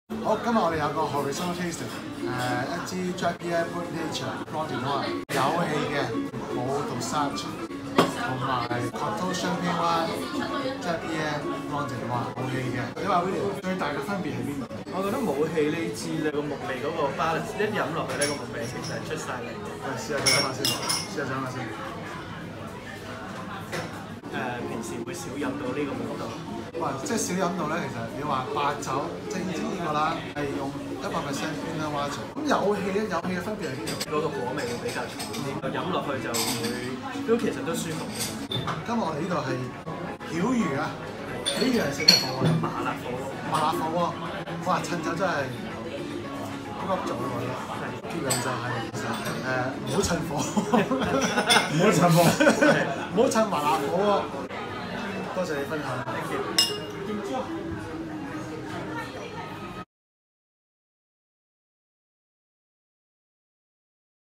EveryDayFineWine60sJournal （14） - Horizontal Tasting on two Champagne from Champagne Drappier
*Champagne Drappier Blanc de Noir Brut Nature NV
*Champagne Drappier Coteaux Champenois Blanc de Noir NV (non-sparkling white)

熟成酒窖主理人Dennis致力研究陳年葡萄酒的適飲期限，藉着用此Video Blog和wine lovers分享經驗。特別嗚謝 William 品酒分享及Danny友情拍攝！